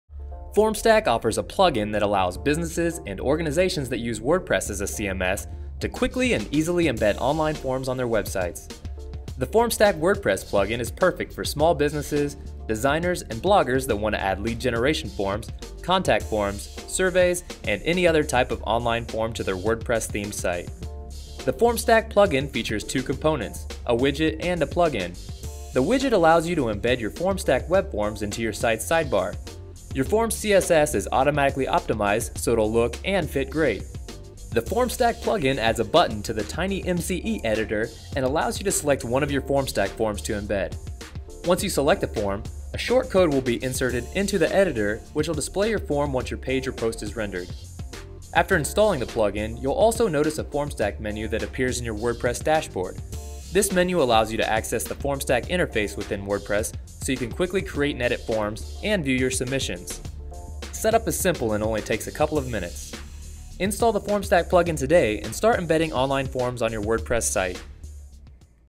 Wordpress Integration
- The Formstack Web Form plug-in for WordPress makes embedding your Formstack forms as easy as clicking a button.

This update offers a tighter integration with Formstack and provides you easy access to your forms, the form builder, and submissions directly inside your WordPress admin interface.

This should also help fix some of the nagging compatibility issues some users have experienced with earlier versions of our WordPress plug-in.